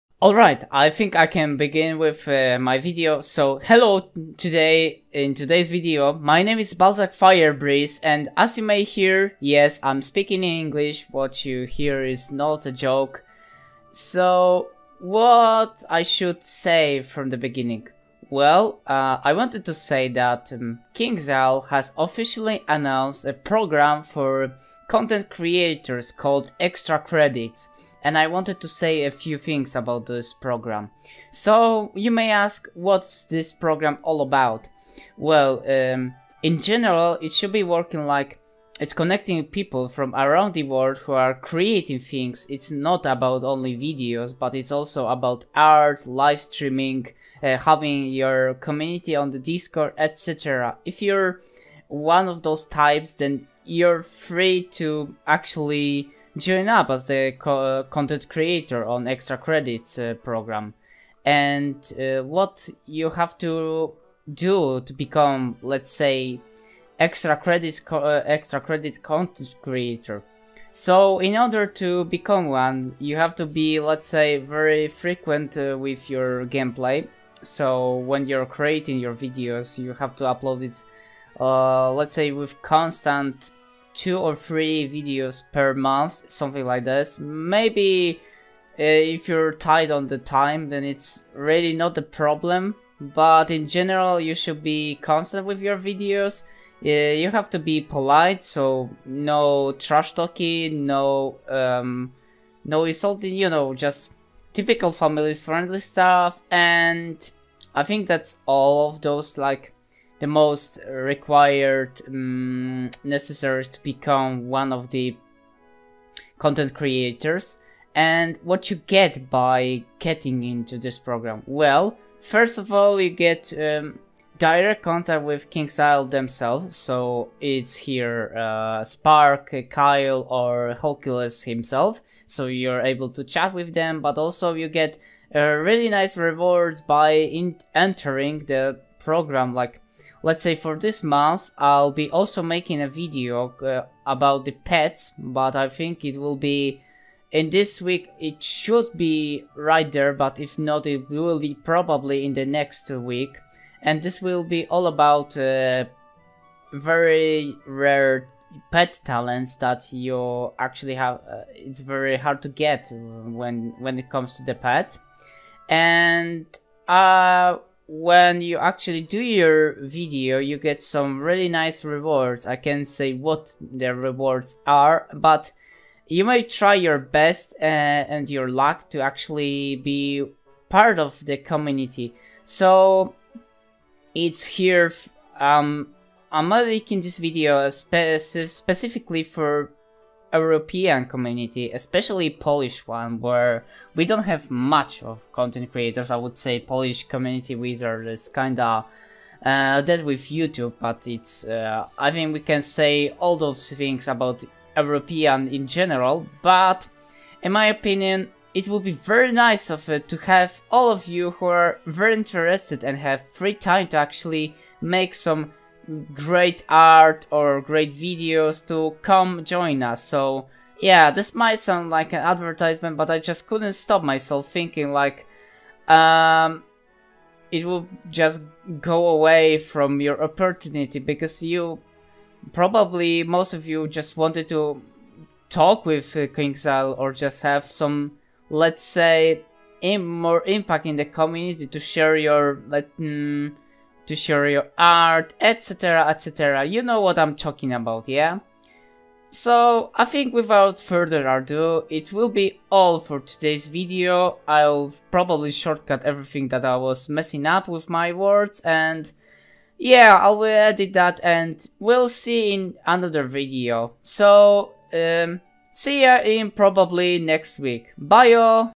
Extra Credits Programme
Hello my name is Balsac Fire Breeze and in today's video I will briefly talk about latest programme for content creators from Ki themselves :)! I will tell you what's this programme all about it, what are the requirements to join and what do you get by joining the programme itself. So without further ado enjoy :).